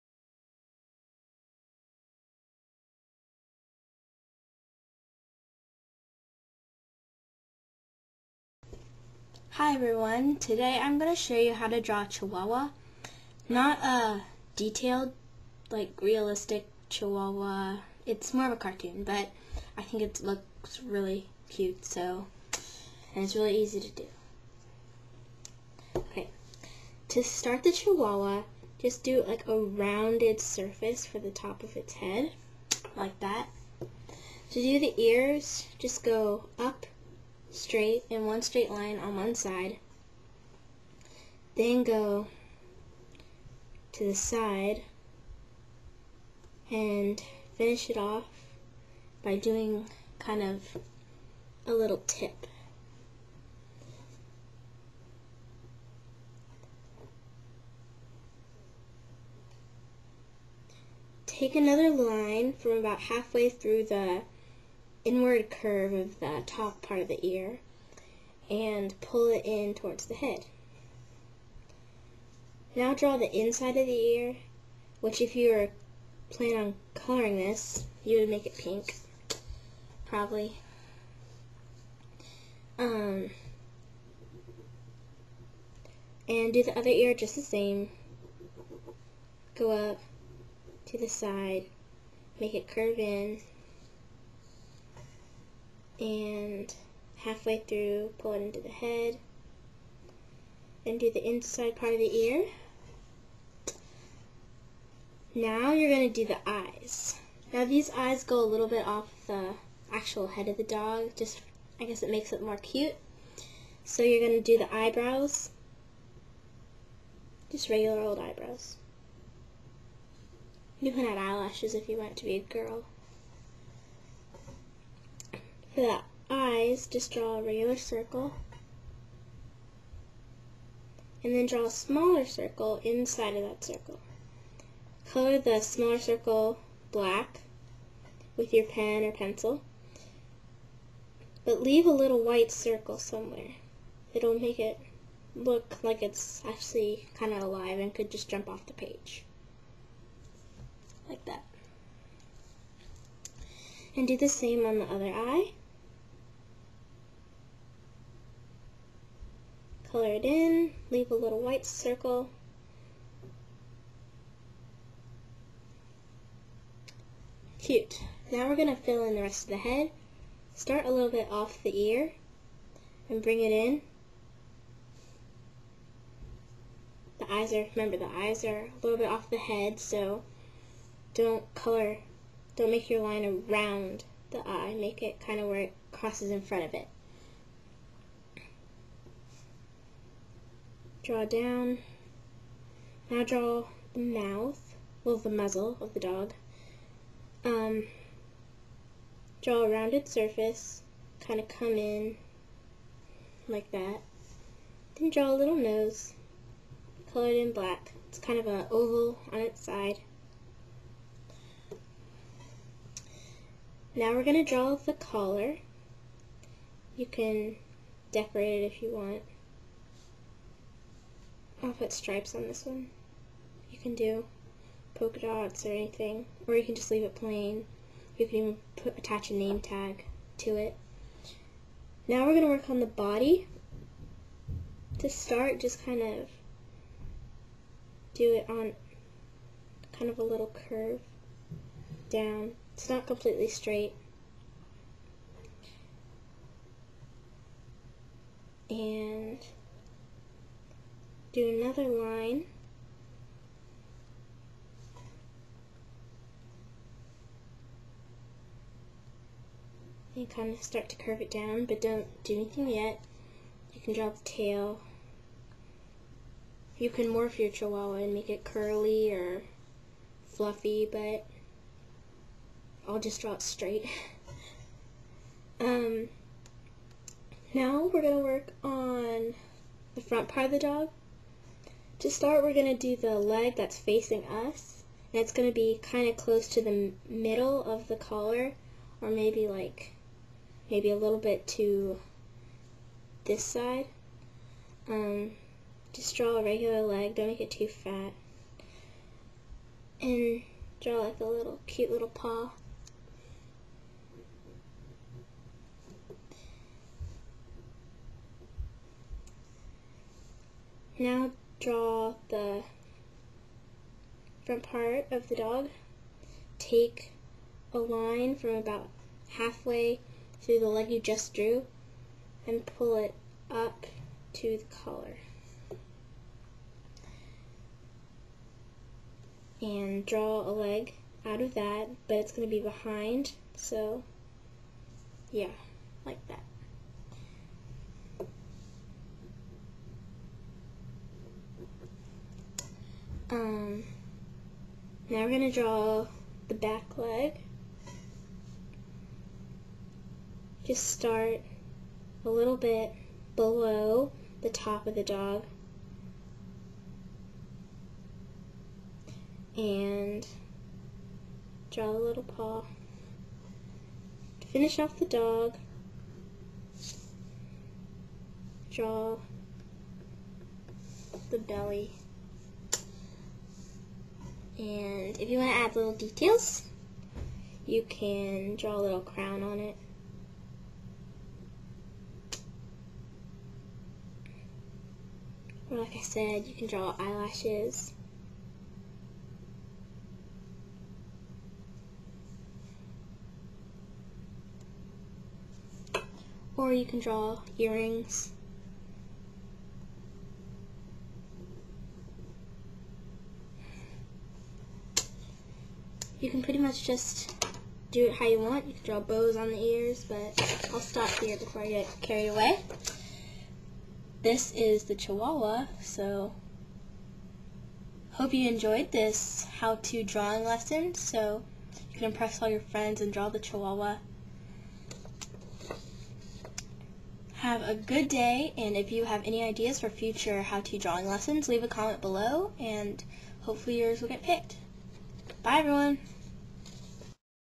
How To Draw a Chihuahua
This is a tougher draw folks, but not impossible. learn in under 8 minutes.